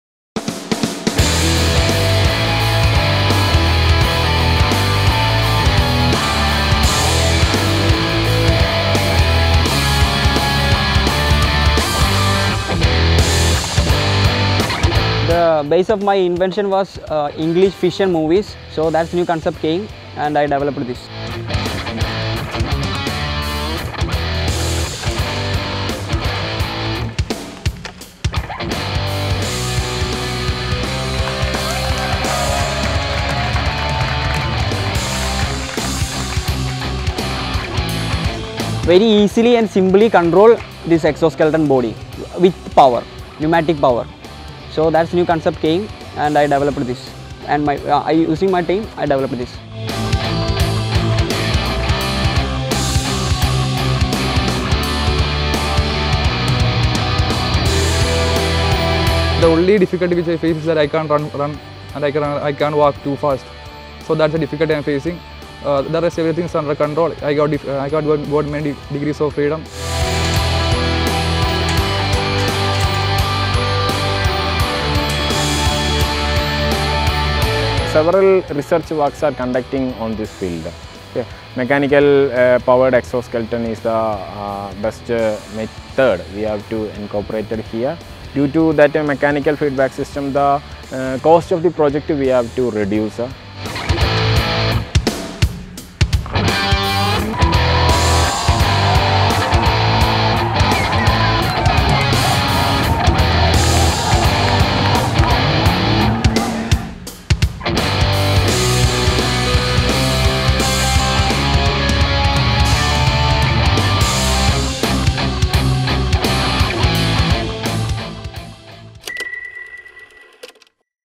The Quint| Check it Out: Indian Students Engineer This Cool ‘Iron Man’ Suit
This exoskeleton is the brainchild of final-year student, Vimal Govind Manikandan from the MES College of Engineering.
He says he was inspired by Hollywood movies, especially Avatar’s fictional AMP (Amplified Mobility Platform) suit.
Ditching expensive and complex software programs and sensors, Manikandan says his G2 prototype uses battery-powered pressurized air-chambers that help the giant robot move about and lift up to 150 kilograms.

Developed with help from college authorities and students, the G2 uses a pneumatic compressor unit - or pressurized-air chamber system - and a control unit which combine to make the prototype workable.
The team of students designed and built the frame with local support.
Manikandan believes his robot can potentially be used in the military and industrial sectors which require the lifting and moving of heavy materials.
Though not as sleek as similar products on the market, like the recently unveiled range of exoskeleton suits by Panasonic, the G2 has one big advantage.
It cost just US$750 to build the prototype.